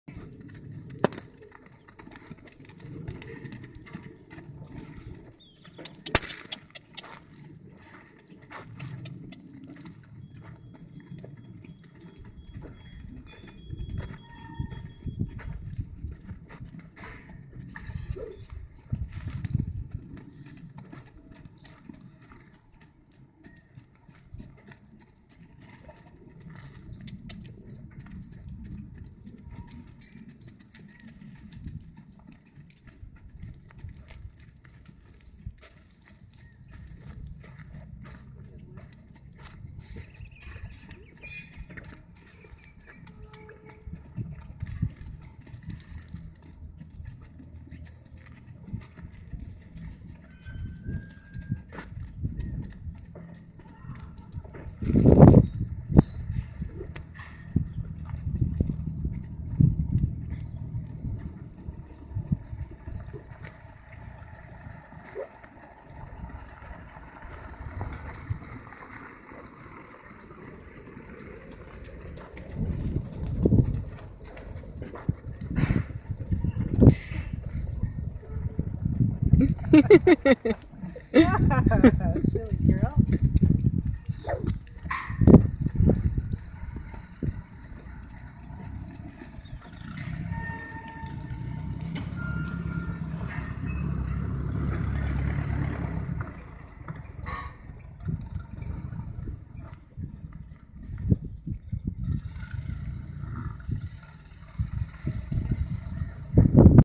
RMS Lady's Godiva half Friesian filly for sale
Buckskin Half Friesian filly for sale in Mesa, AZ.